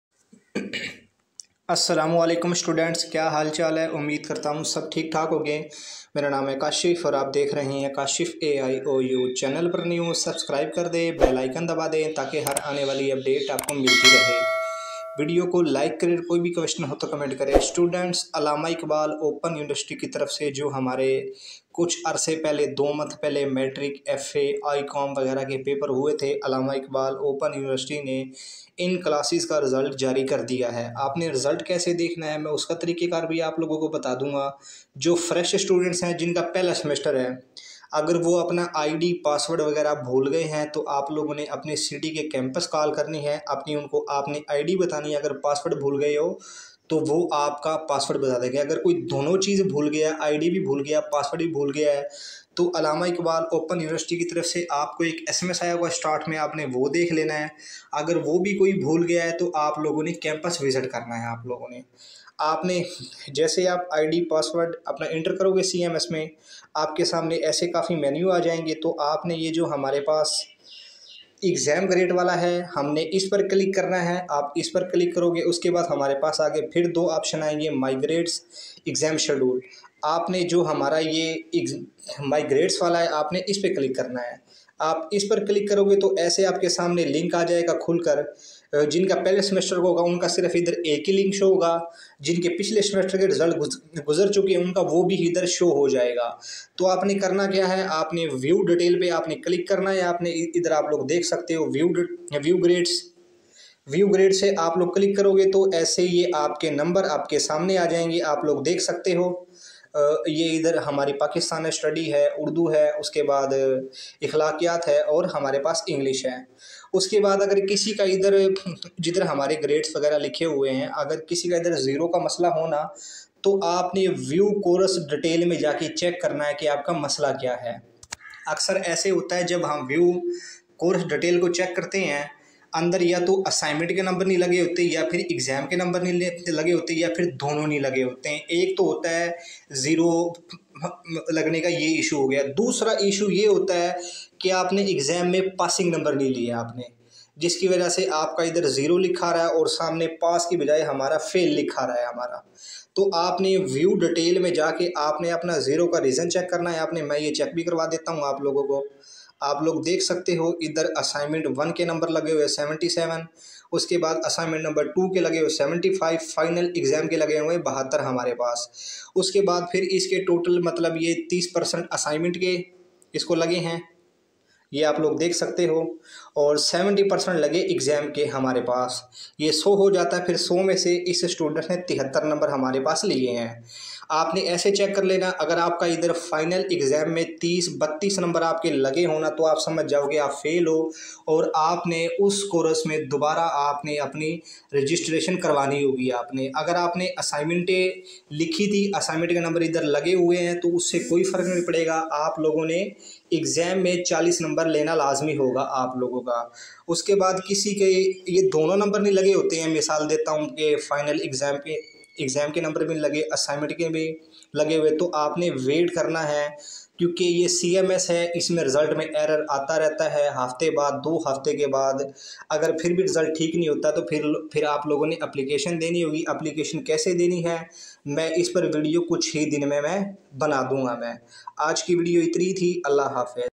Aiou class matric f.a i.com result announced Semester Autumn 2023
Allama Iqbal Open University Class Matric f.a i.com Result Announce Semester Autumn 2023 aiou matric f.a i.com Method to check result